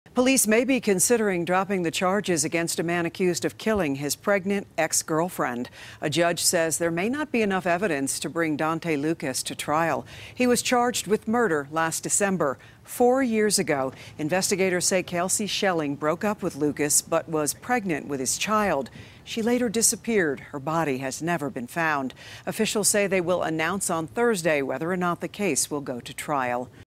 Judge To Rule On Case Against Donthe Lucas
Charges against a Pueblo man accused of murdering his pregnant ex-girlfriend could be dropped in the coming days.